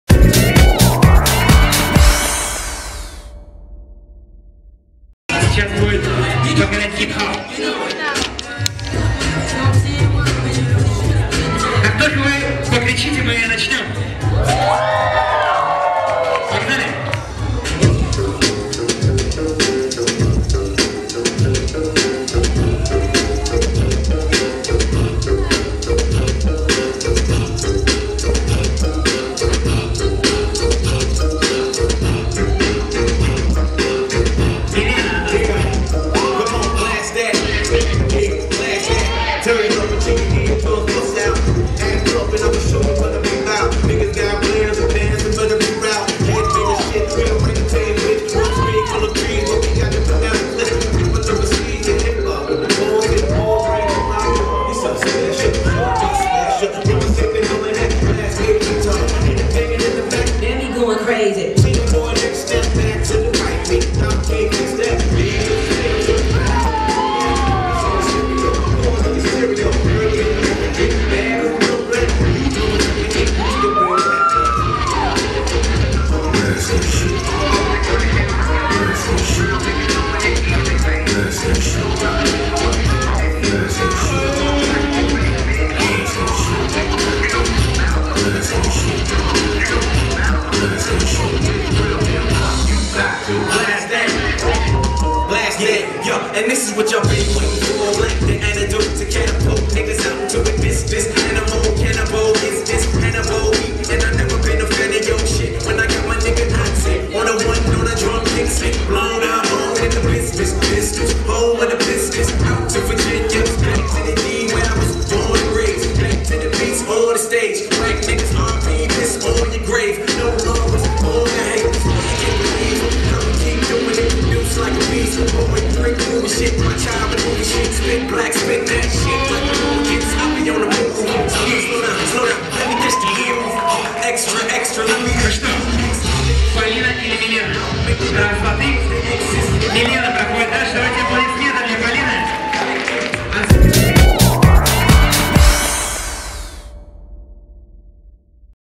Уроки брейк данса для детей
Танцевальный батл по Hip-Hop и Breaking среди детей и подростков прошел 14 февраля в Москве. Батл организован танцевальной и театральной школой для детей "Ирбис".